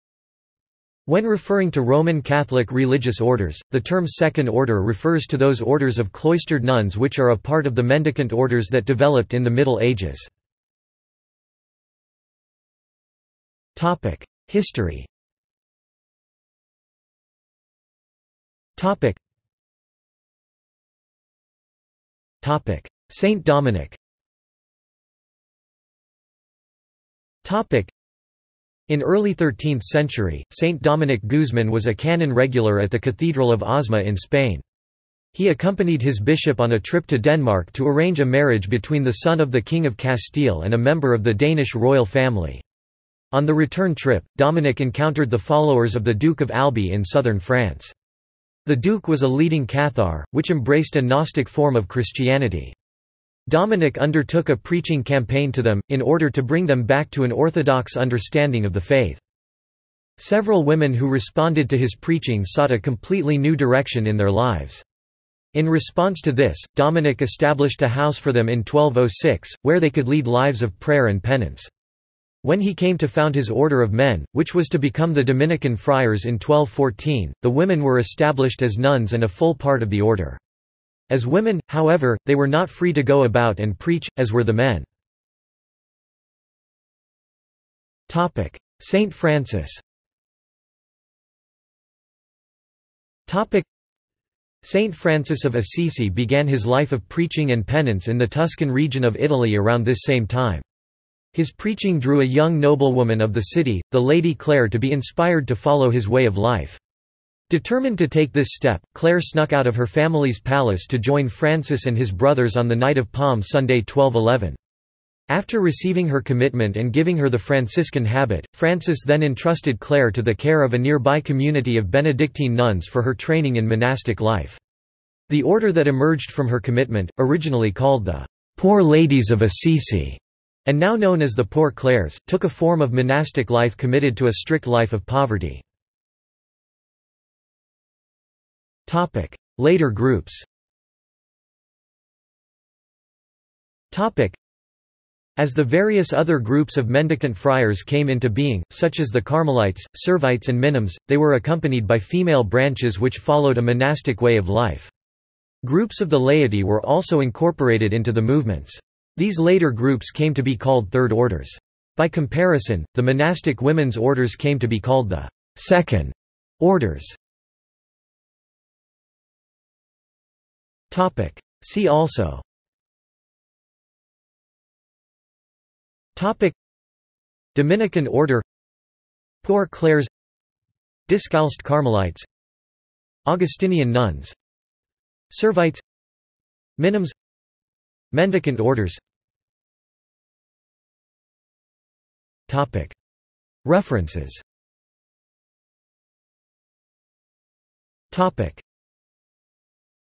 Second Order (religious) | Wikipedia audio article
This is an audio version of the Wikipedia Article:
Second Order (religious)

SUMMARY
When referring to Roman Catholic religious orders, the term Second Order refers to those Orders of cloistered nuns which are a part of the mendicant Orders that developed in the Middle Ages.